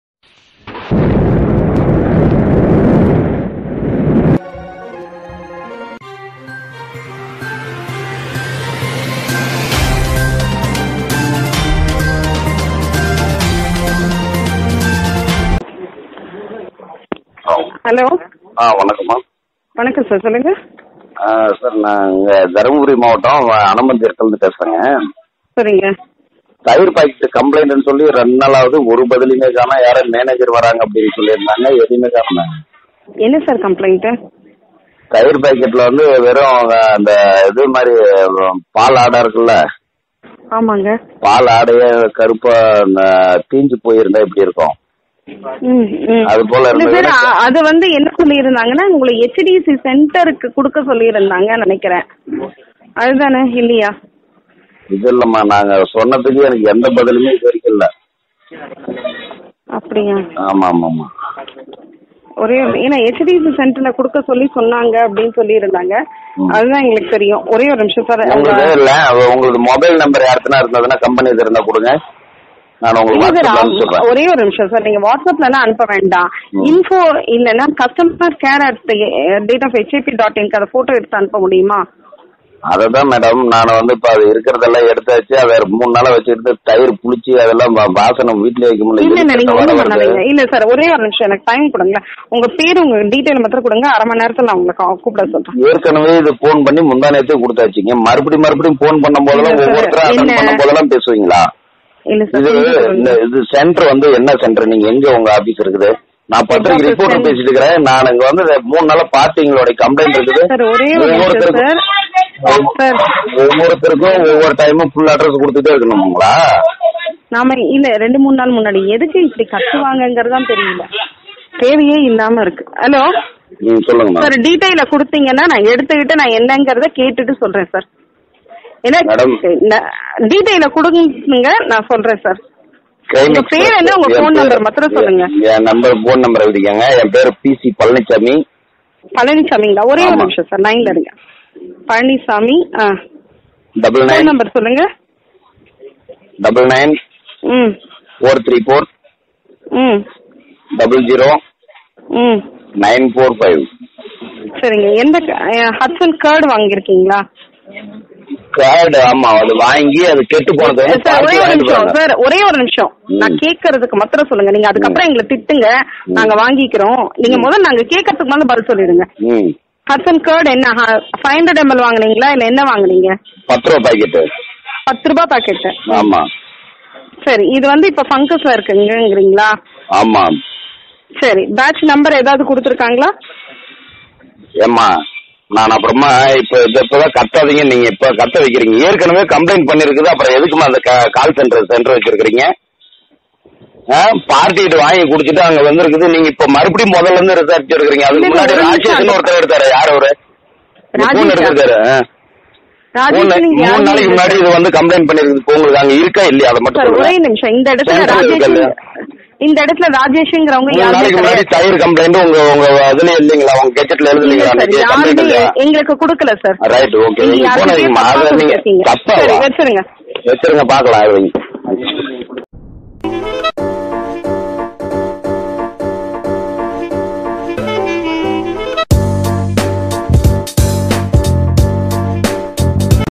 KALAM NEWS தர்மபுரி மாவட்டம் அனுமந்தீர்தம் பகுதியில் கெட்டுப்போன ஆட்ஸன் டைரி தயிர் பாக்கெட் கேள்வி க
KALAM NEWS
தர்மபுரி மாவட்டம் அனுமந்தீர்தம் பகுதியில் கெட்டுப்போன ஆட்ஸன் டைரி தயிர் பாக்கெட் கேள்வி கேட்ட வாடிக்கையாளரை தரம் தாழ்ந்து பேசிய பெண் ஊழியர்